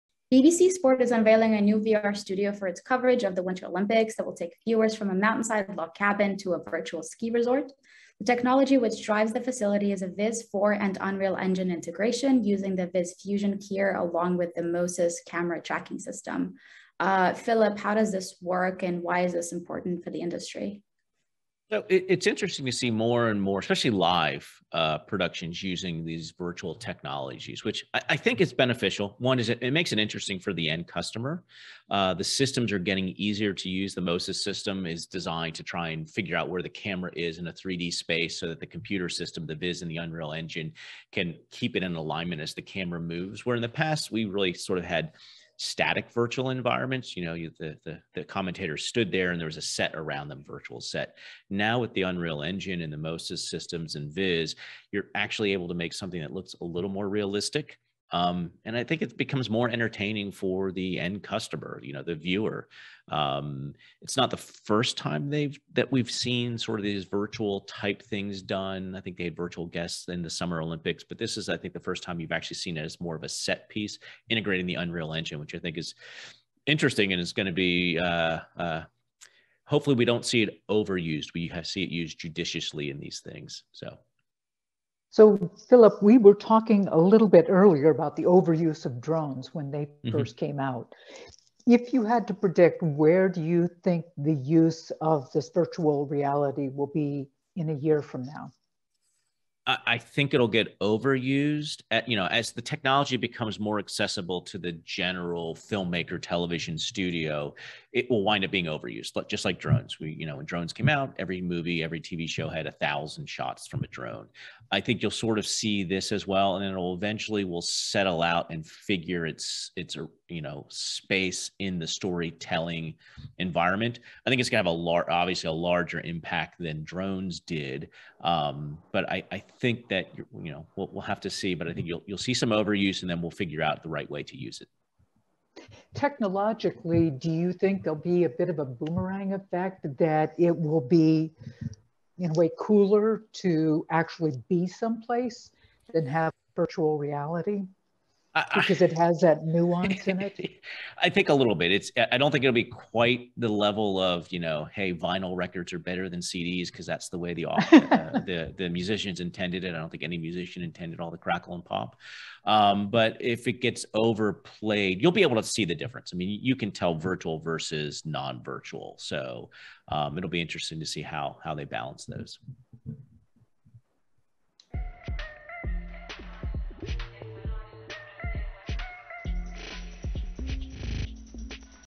BBC Sport: the innovative new VR studio for Beijing Winter Olympics/ 360 seconds. News & Commentary
BBC Sport is unveiling a new VR studio for its coverage of the Beijing Winter Olympics that will take viewers from a mountainside log cabin to a virtual ski resort. The technology which drives the facility is a Viz 4 and Unreal Engine integration using the Viz Fusion keyer along with the Mo-Sys camera tracking system.